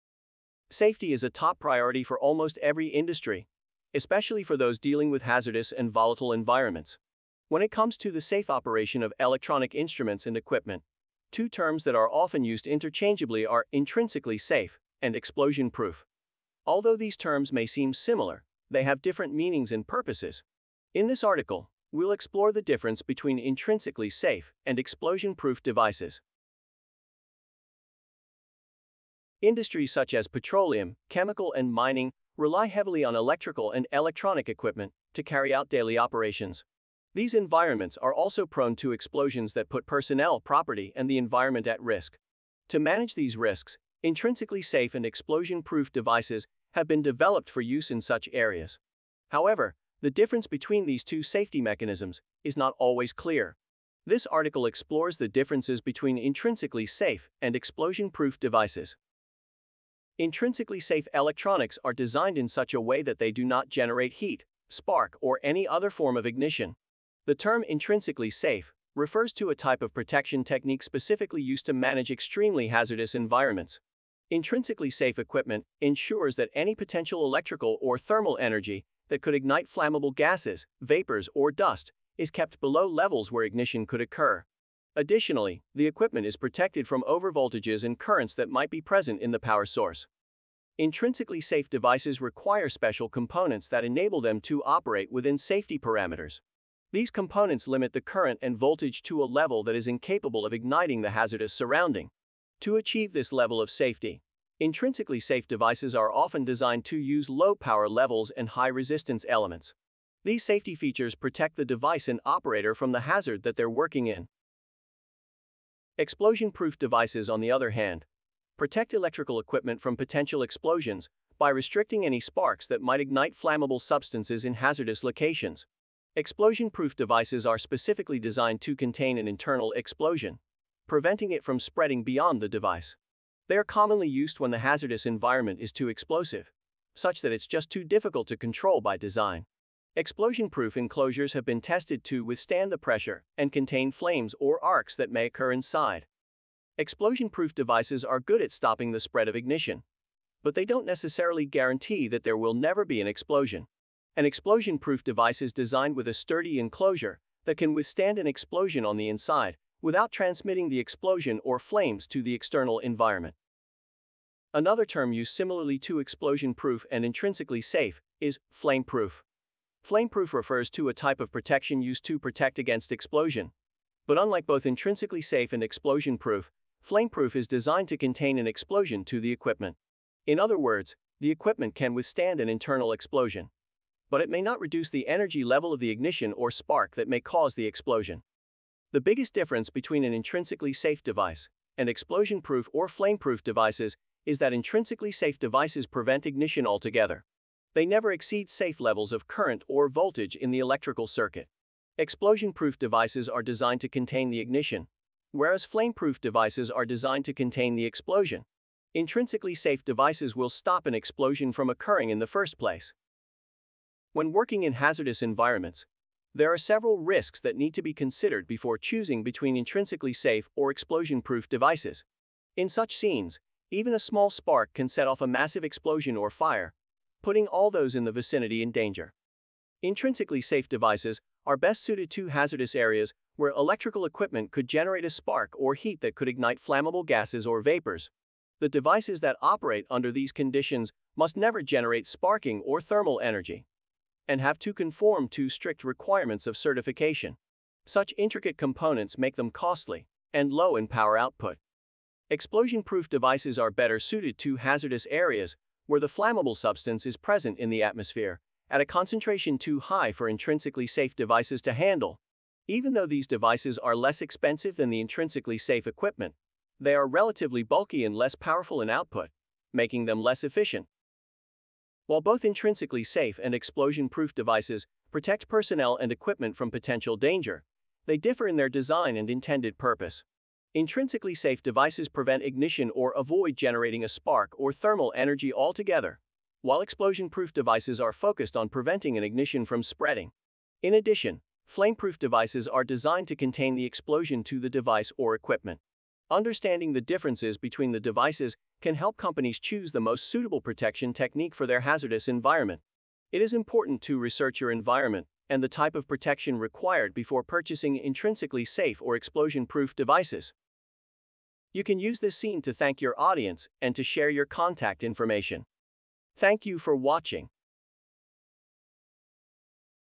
What Is The Difference Between Intrinsically Safe And Explosion Proof
Intrinsically safe and explosion-proof devices are two safety mechanisms used in hazardous environments. Intrinsically safe devices prevent ignition, while explosion-proof devices contain any ignition that may occur.  The article explains the differences between the two and gives recommendations on the best safety measure to adopt for specific environments.